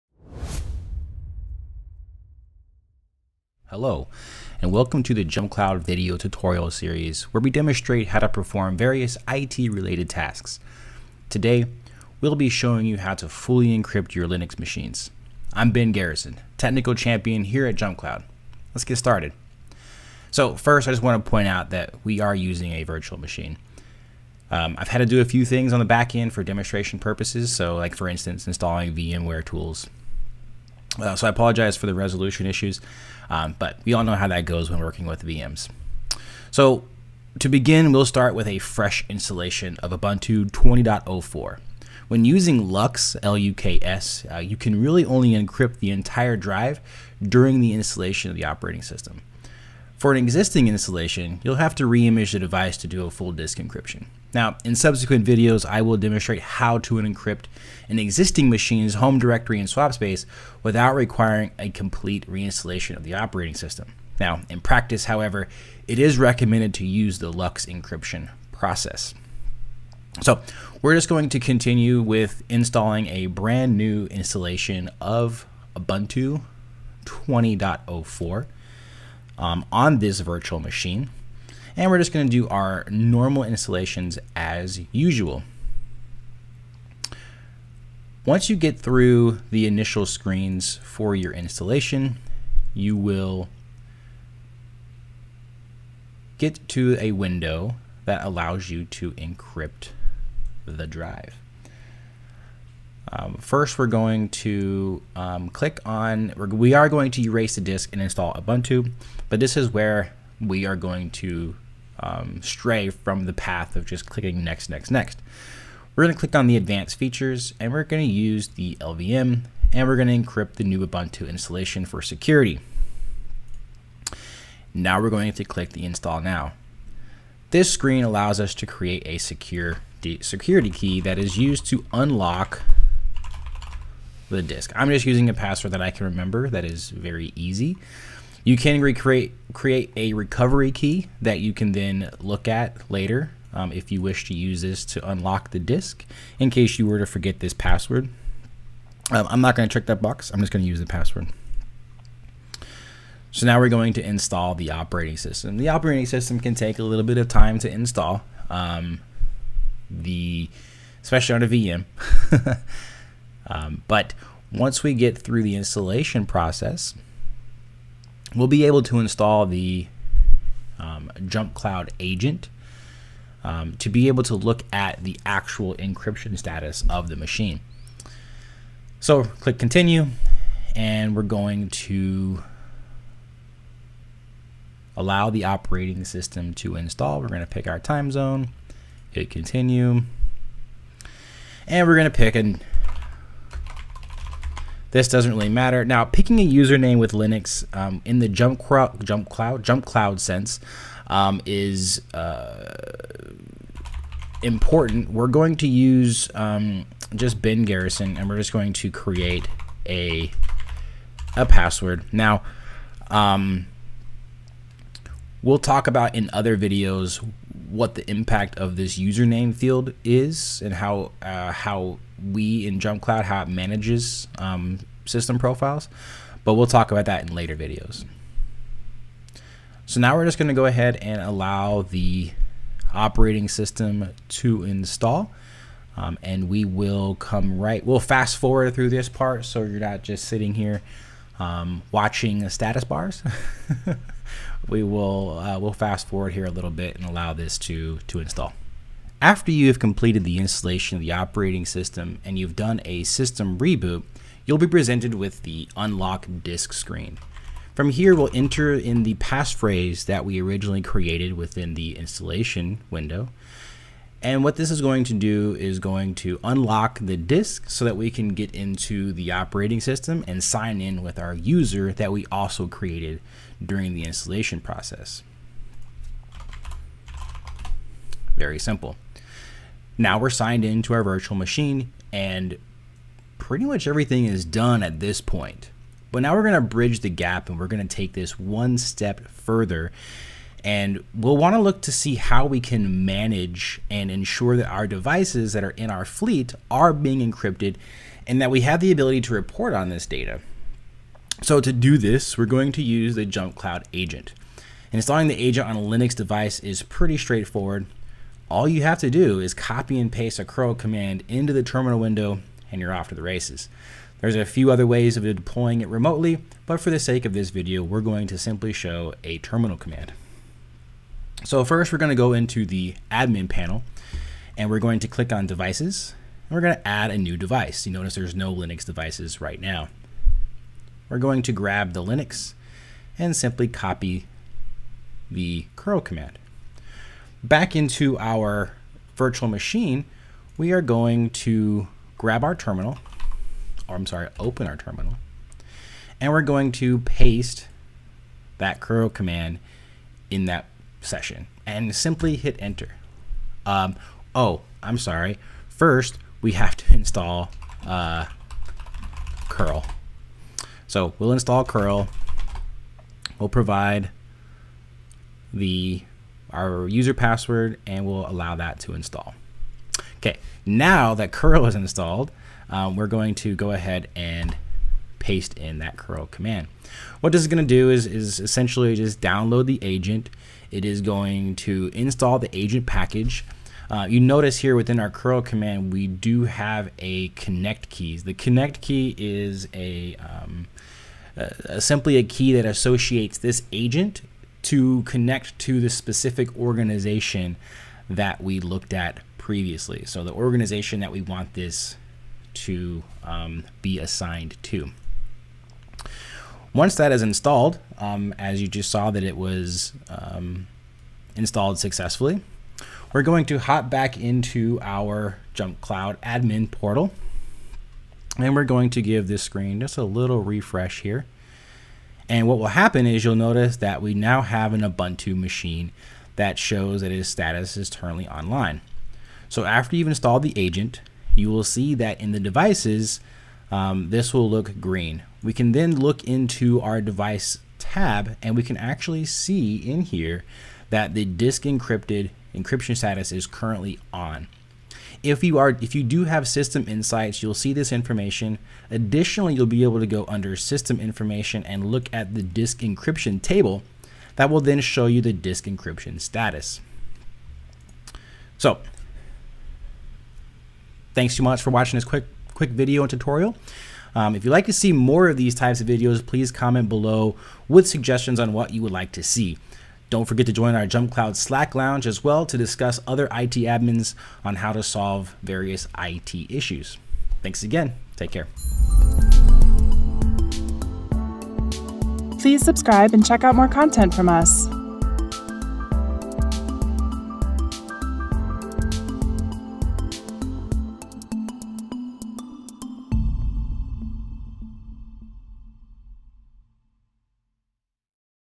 Ubuntu 20.04 Tutorial: Ubuntu Full Disk Encryption Install With LUKS
Learn how to perform a Ubuntu full disk encryption install using Linux Unified Key Setup (LUKS) with Ben Garrison, JumpCloud’s Technical Champion. Read the full text walkthrough

With remote work becoming the new normal and cyber threats becoming more powerful and sophisticated, enabling full disk encryption on Linux machines using LUKS is one effective way to keep data on physical devices secure. LUKS is free, provides complete encryption, and allows users to manage numerous keys and passwords.

Note that if you are using LUKS to encrypt a Linux machine, full hard drive or SSD encryption can only be performed during the installation process. This means that you will need to reinstall Ubuntu if you are currently running it and would like to encrypt it. Always be sure to back your files up in a secure location before proceeding with an OS reinstallation.